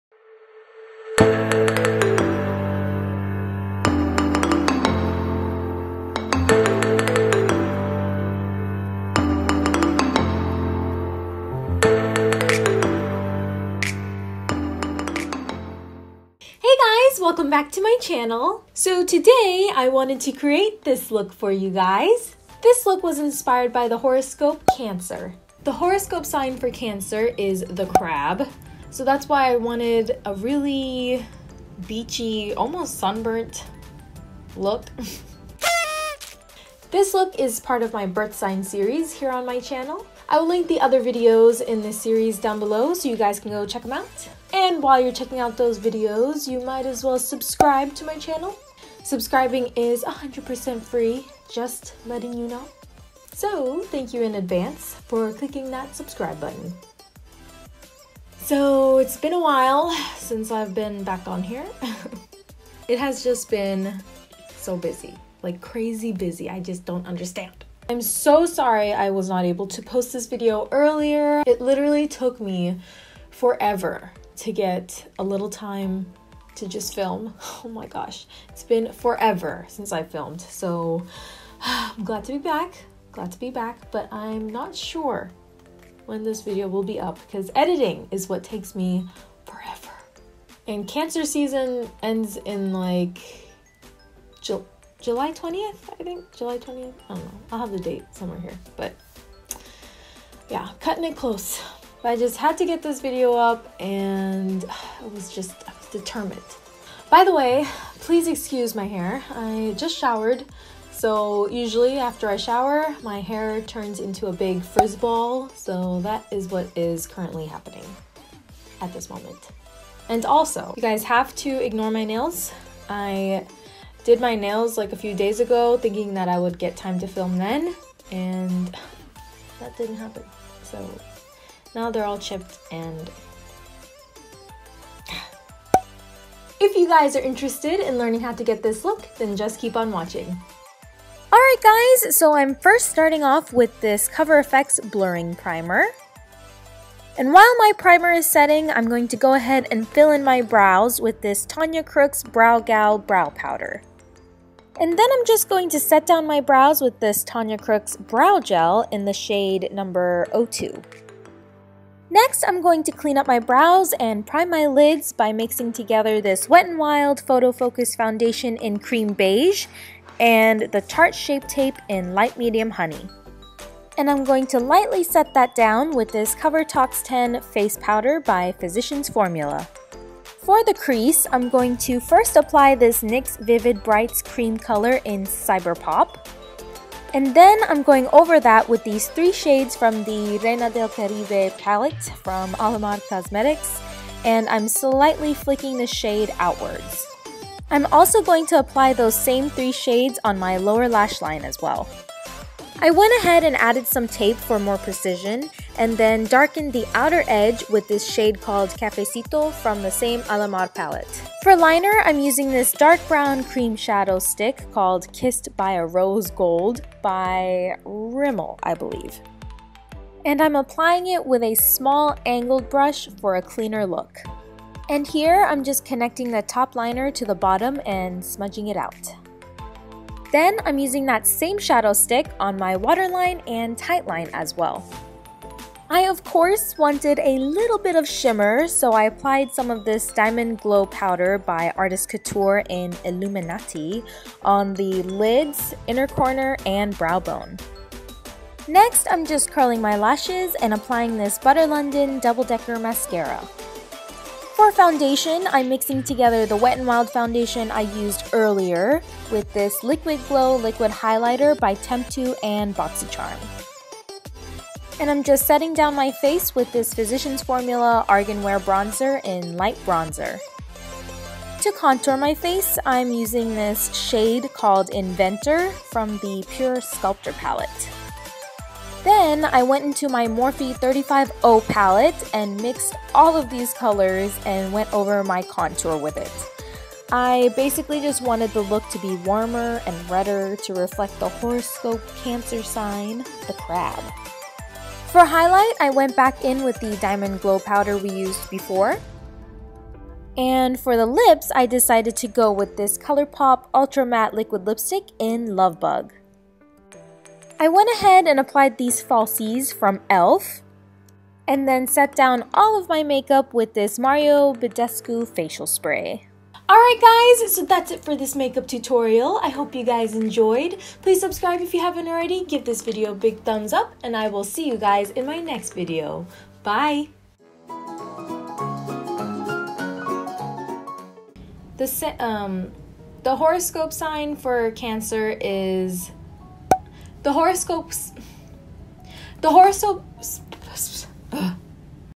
♋Cancer Makeup Tutorial - Birth Sign Series ♋
OPEN ME!!! :)

Here is a look inspired by the birth sign Cancer! Any Cancer babies out there??

What I'm wearing:
Top: JC Penny?